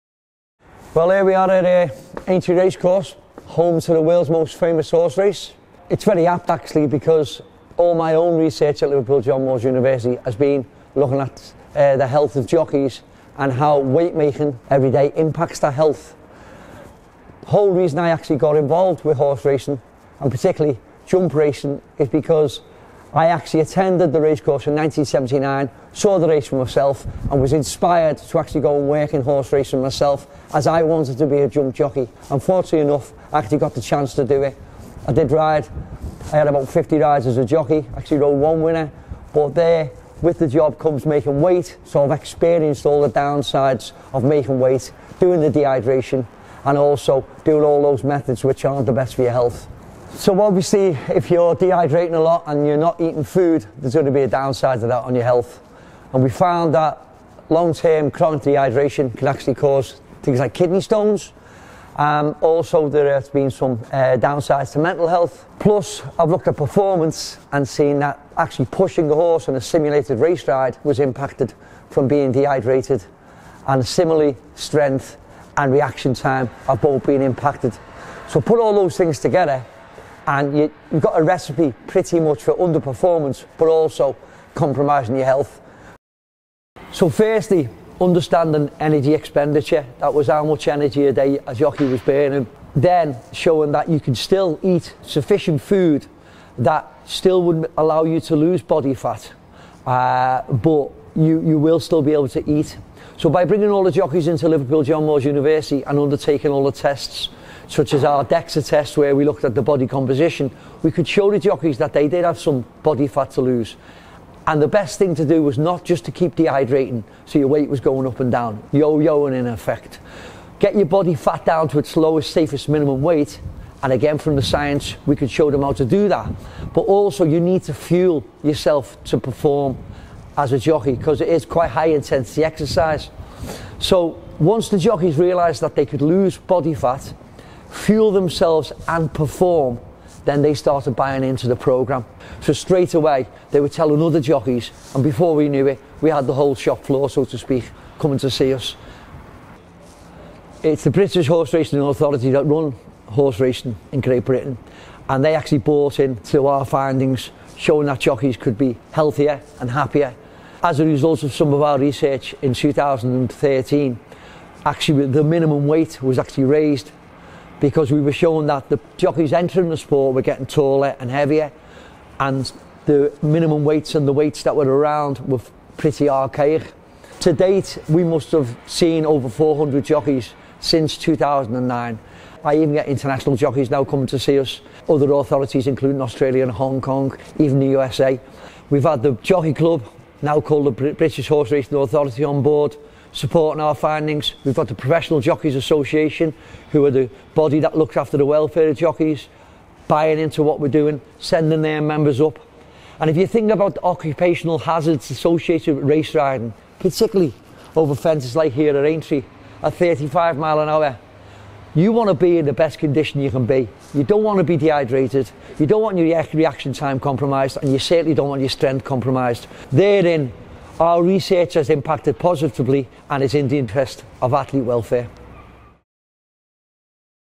How Tailored Nutrition Improves Health and Safety for Jockeys Making Weight - Dr George Wilson
Dr George Wilson, Exercise Physiologist and Sport Nutritionist, has over the past decade led on research projects to better improve the health and wellbeing of jockeys, who are unique athletes in that they have a daily requirement to make weight.

The studies at LJMU have resulted in tailored diet practices being implemented across the sport, and more recently weight increases brought in by the racing authorities have been influenced by the team’s research. All with the aim of promoting better practices that improve both physical and mental health for jockeys not just in the UK, but across the world.